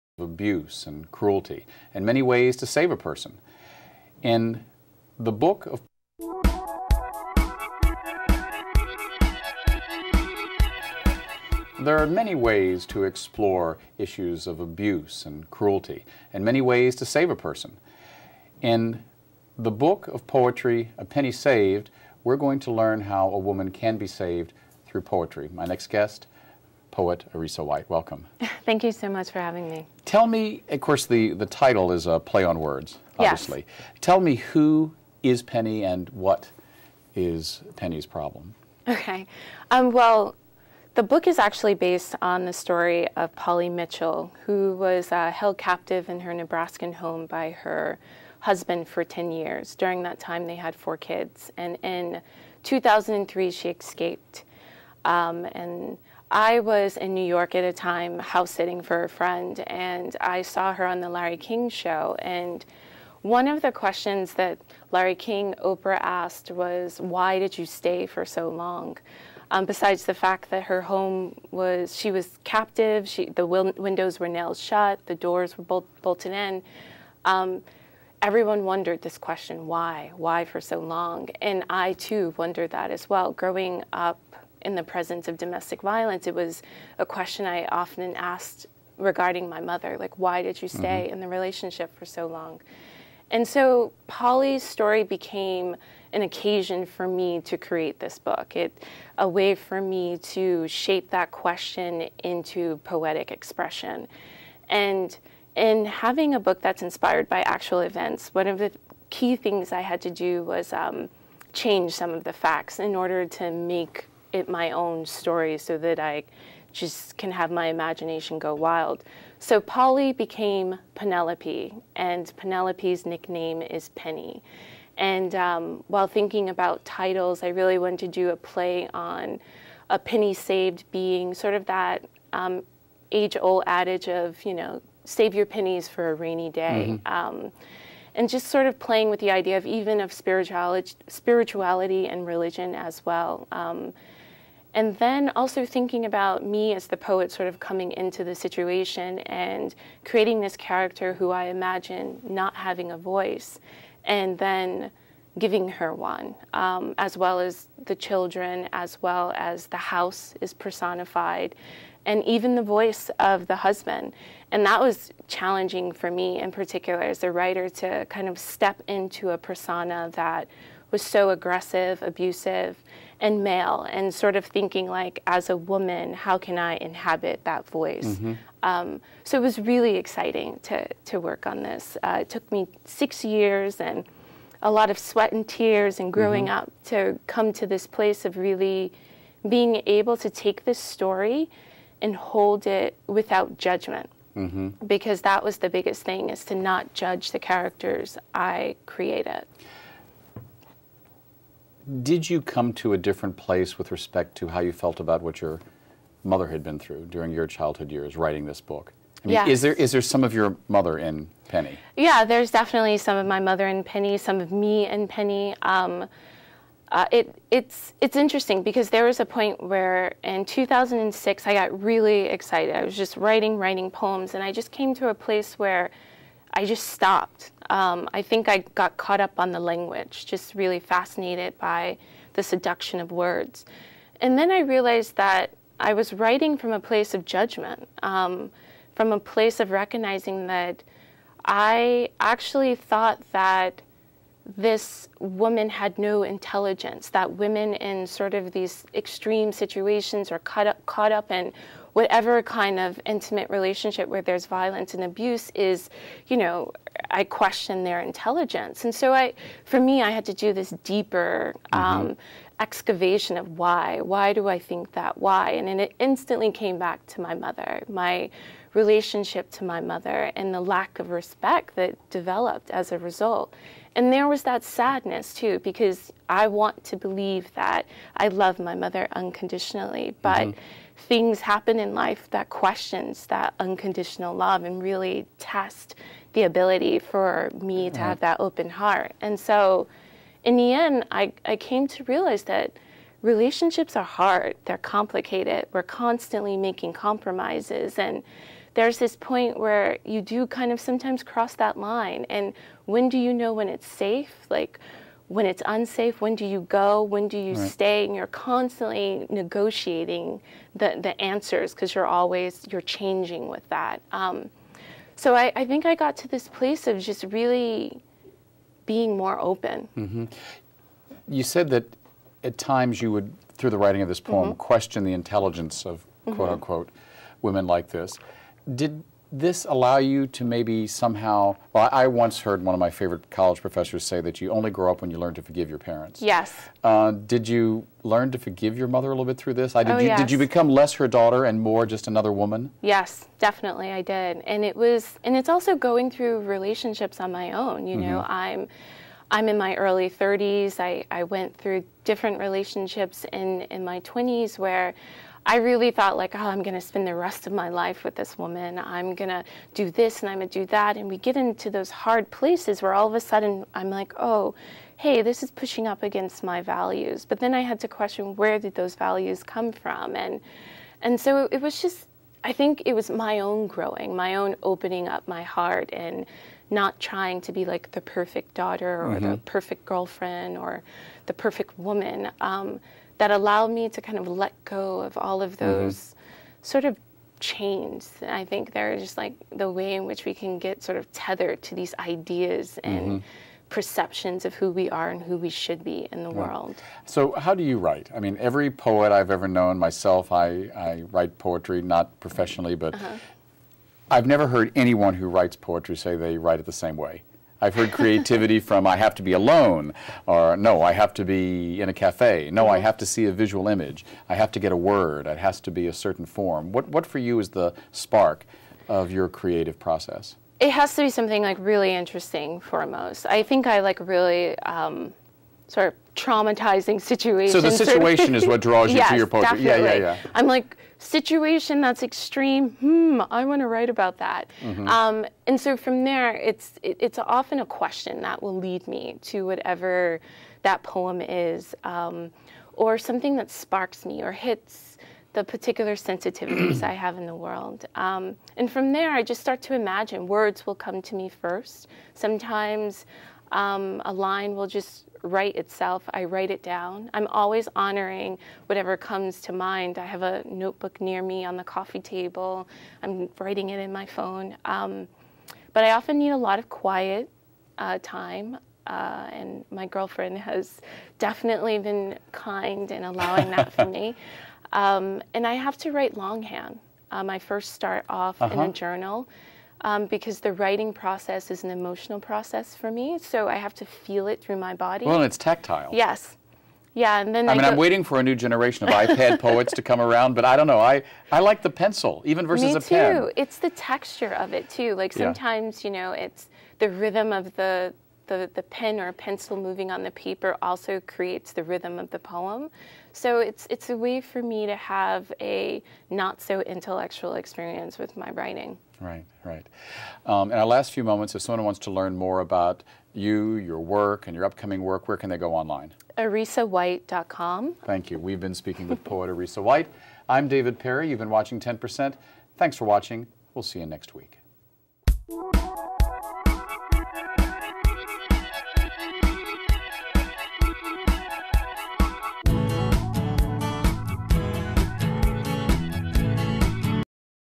David Perry speaks with poet Arisa White about her new collection A Penny Saved
Episode # 163
Monday -- Friday, November 5 - 9 am & 10:30pm
Saturday & Sunday, November 10 - 11, 10:30pm